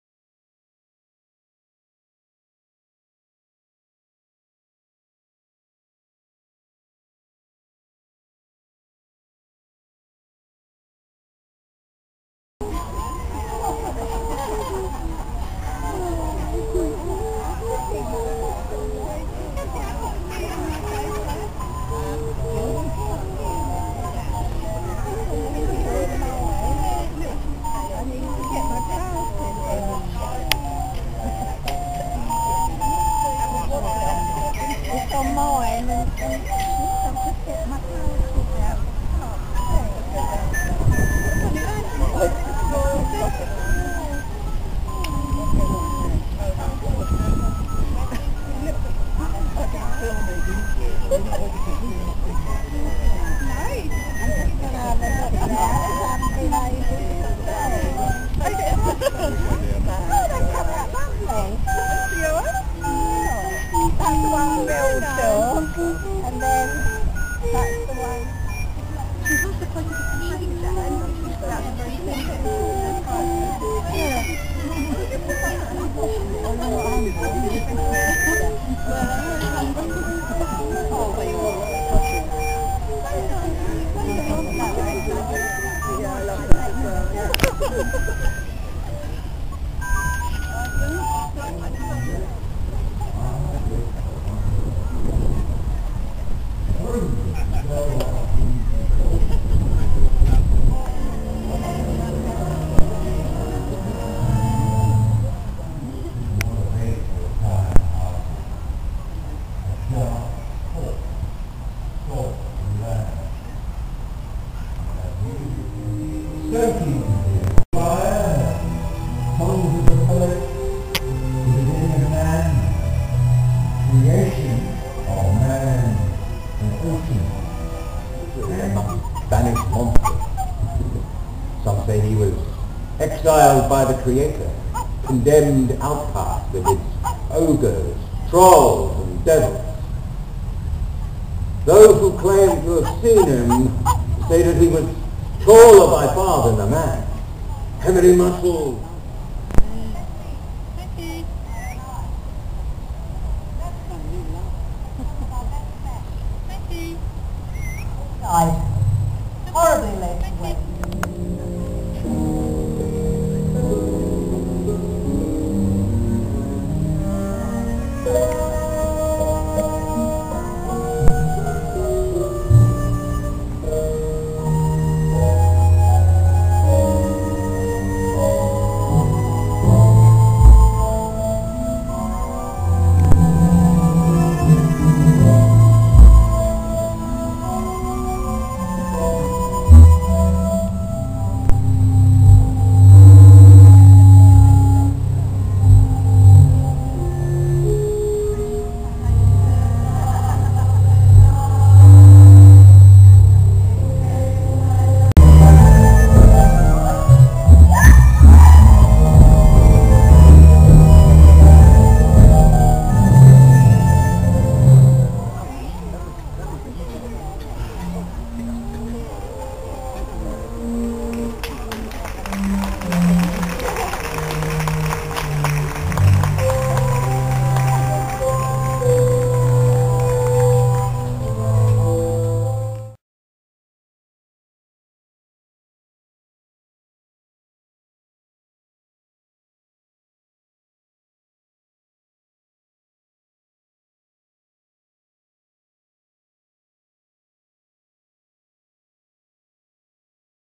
Excerpts Beowulf Final rhsl.wmv
Priory Park Southend on Sea Essex.24th July 2011.Members of the Yardarm Orchestra and the cast of the "Beowulf" production.